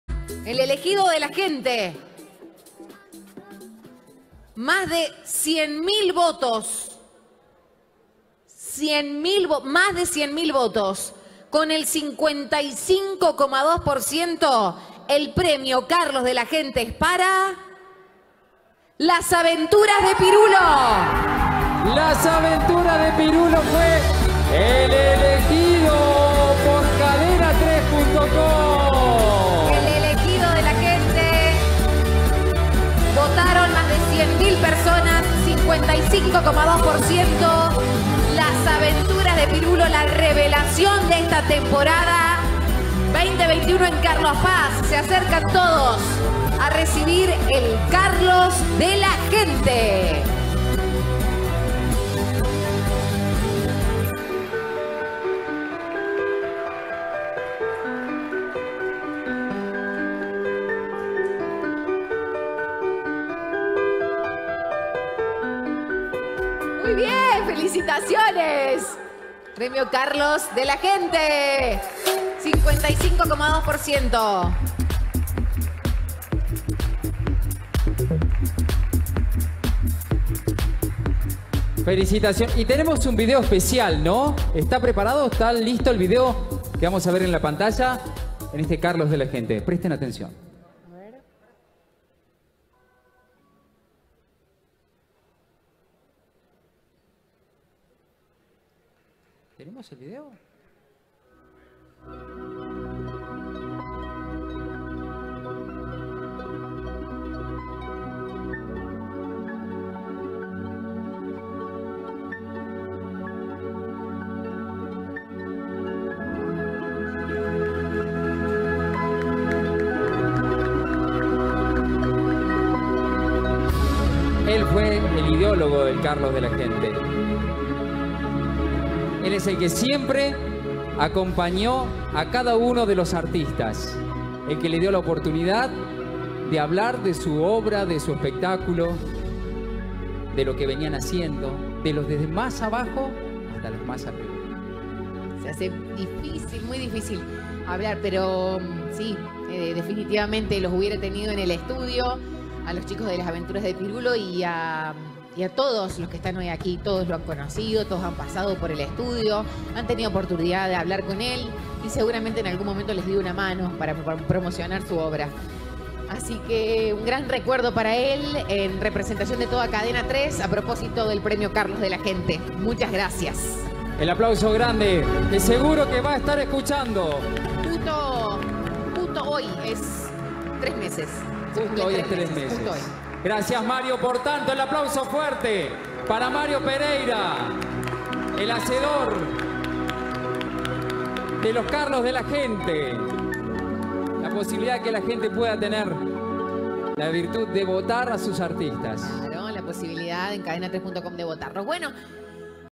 [PREMIOS CARLOS 2021] - EL ELEGÍDO DE LA GENTE, "LAS AVENTURAS DE PIRULO
EL ELEGÍDO DE LA GENTE, "LAS AVENTURAS DE PIRULO", Así recibían la noticia.